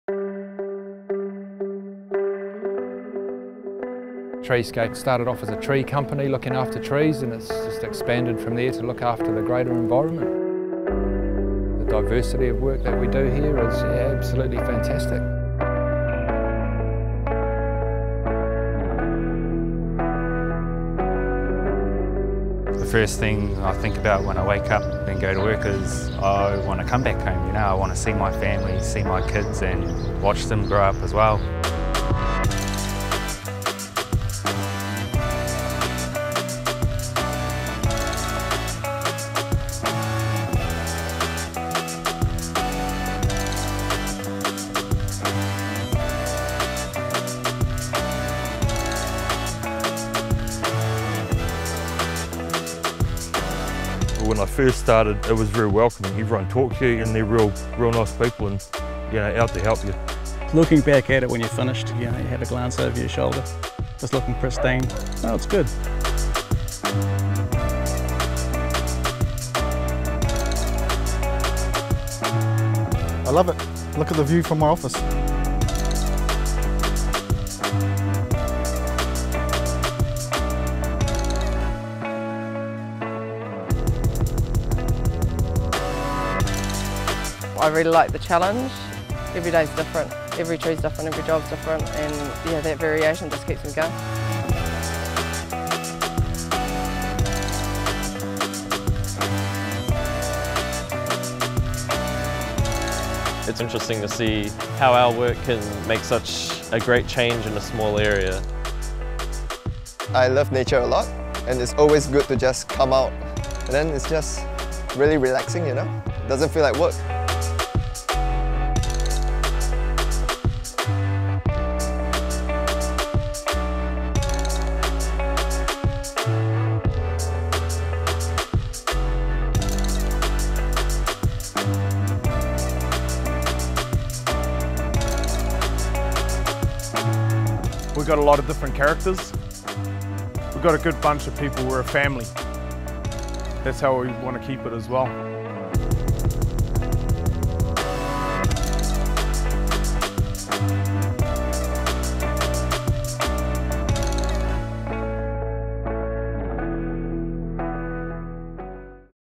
Welcome to Treescape®
Welcome to the world of Treescape® - so much more than cutting down trees! We restore land, protect wildlife, eradicate pests and disease, and transform landscapes on both small and large scales. Want to know more? Watch now!